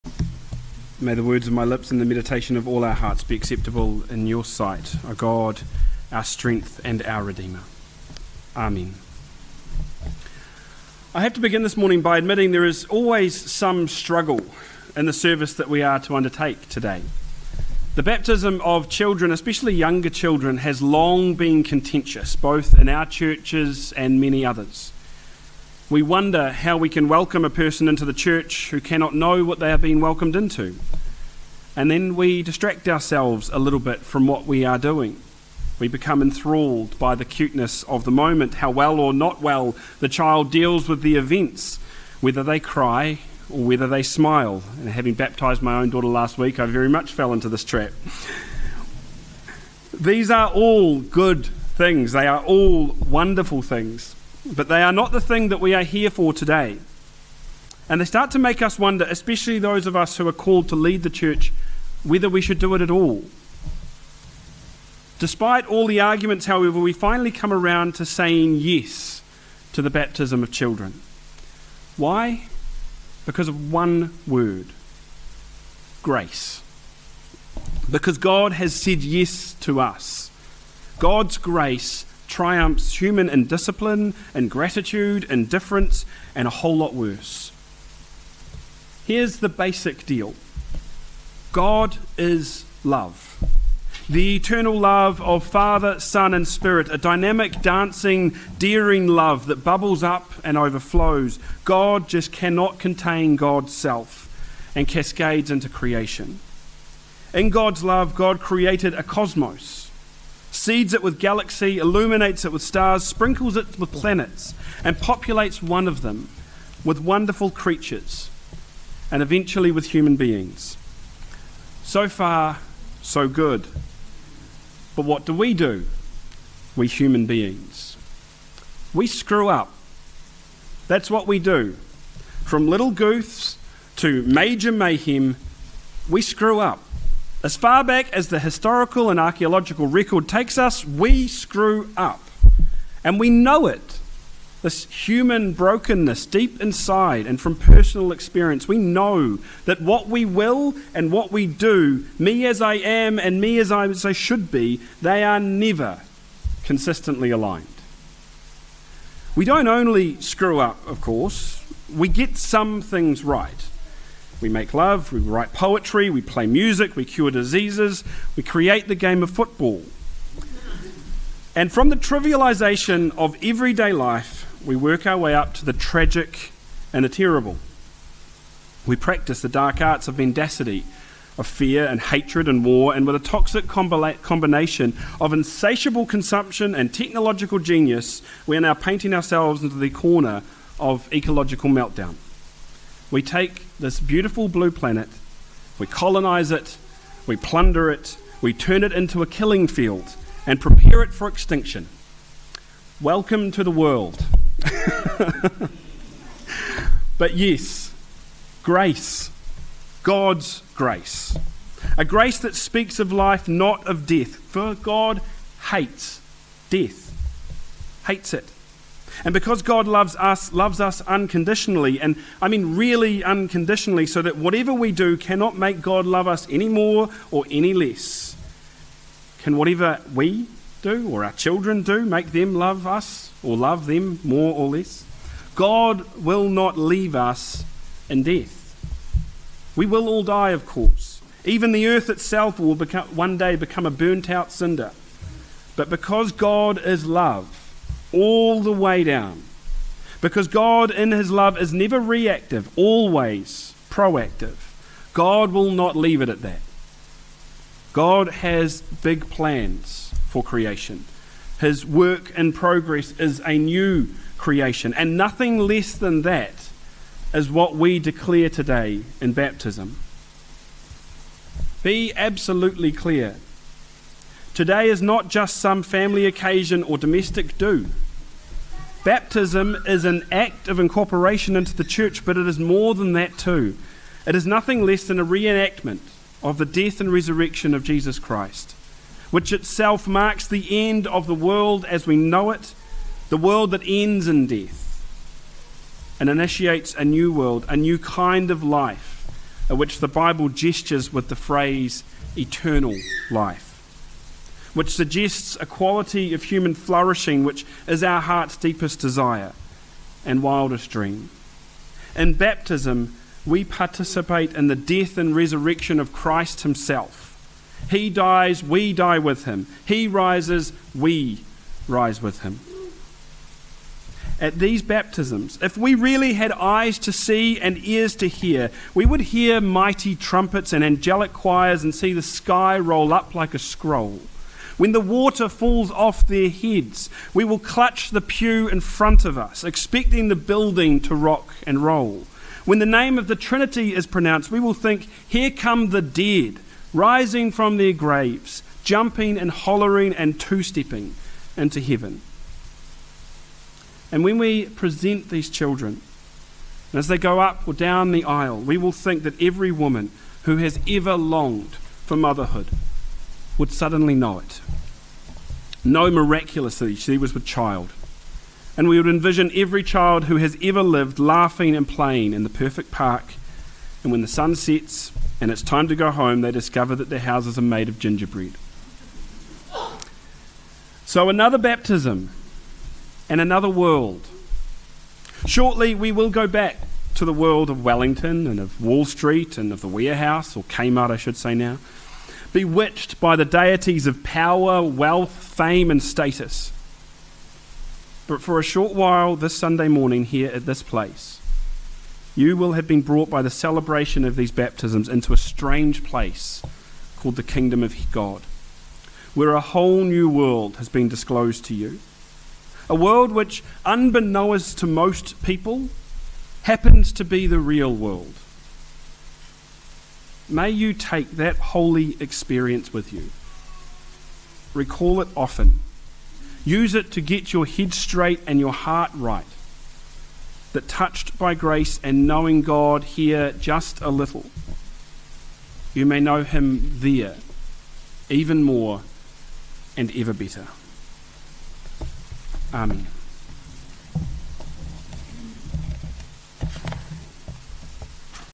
Baptism, God's Grace, and the game of Football
Another Sunday, another Baptism but our message today challenges us to recognise the spiritual truth behind the simple actions we undertake in baptism